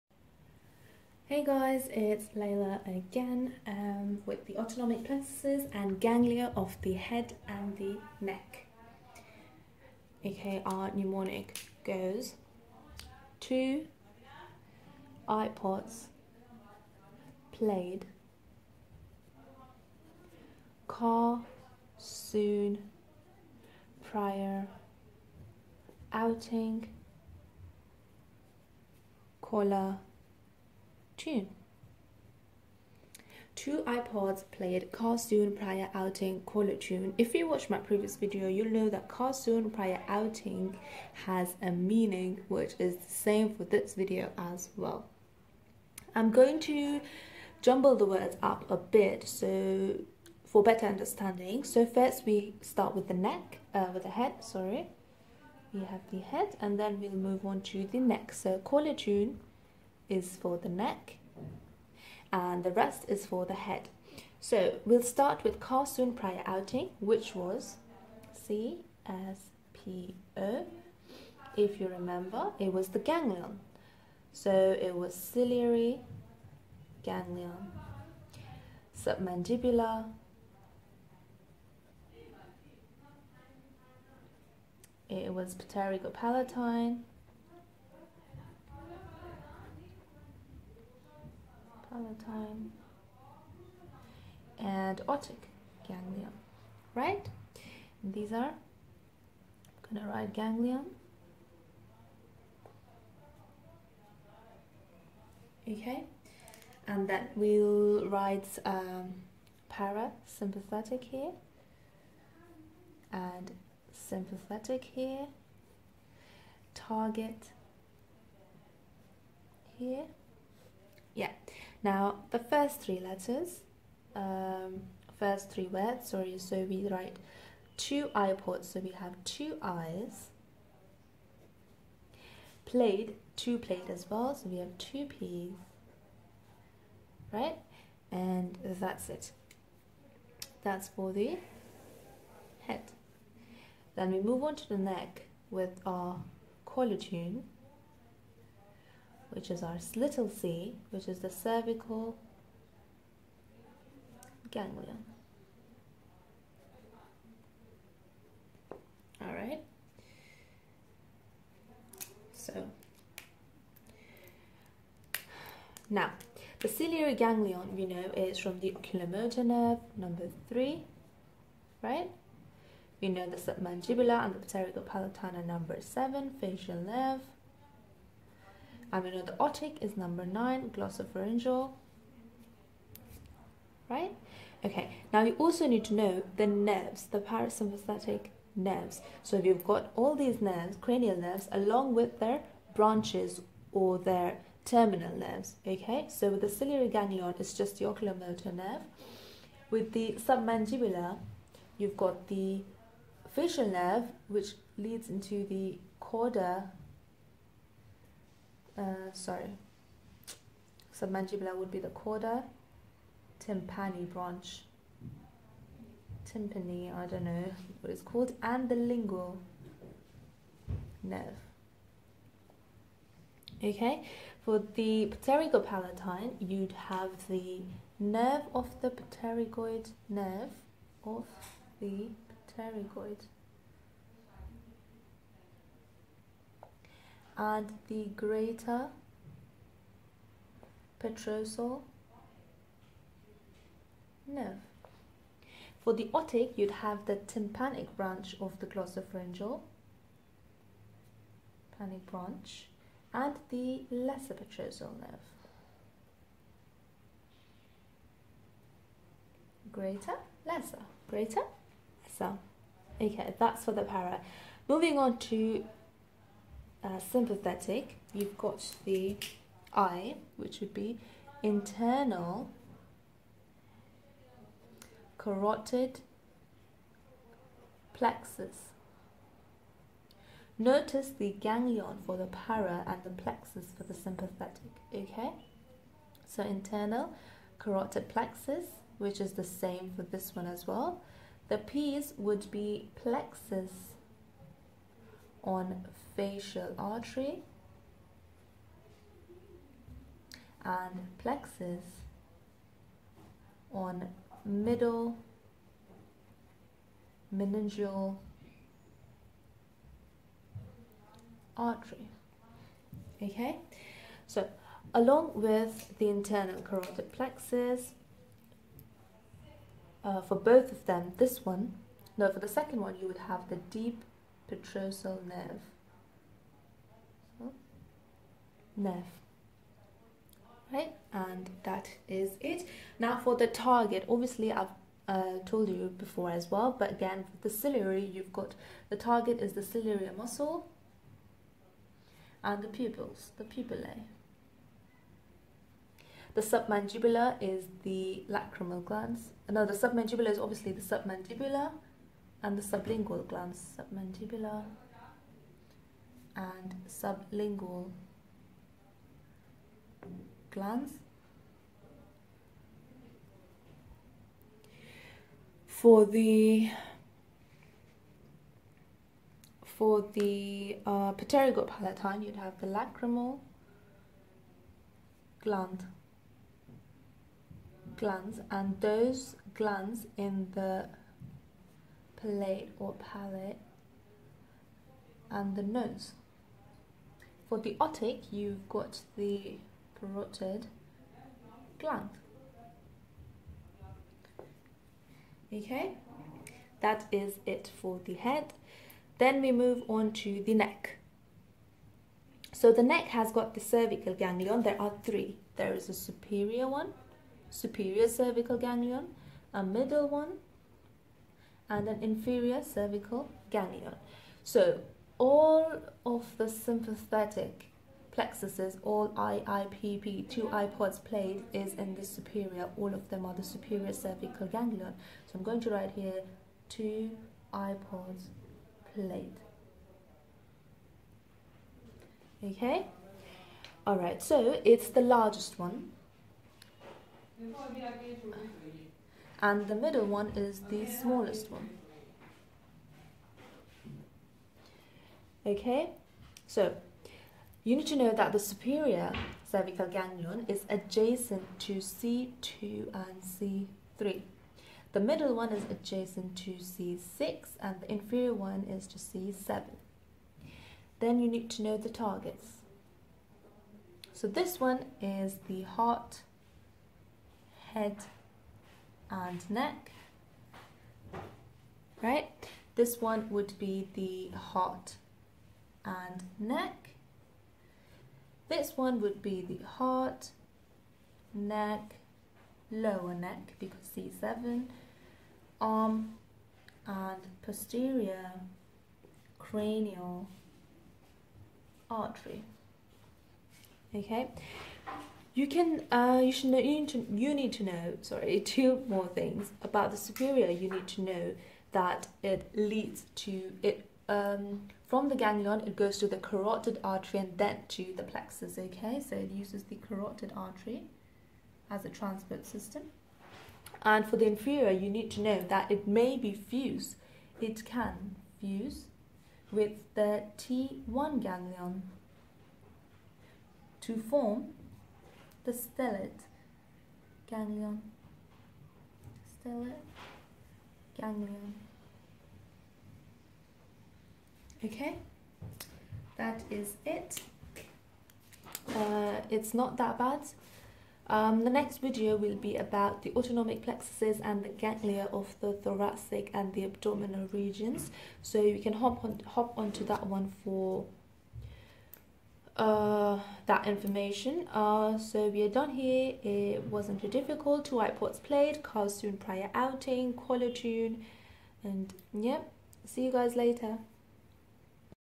Autonomic ganglia: HEAD AND NECK
Learn it the mnemonic way;
2 Ipods played "Car soon, prio outing" caller-tune!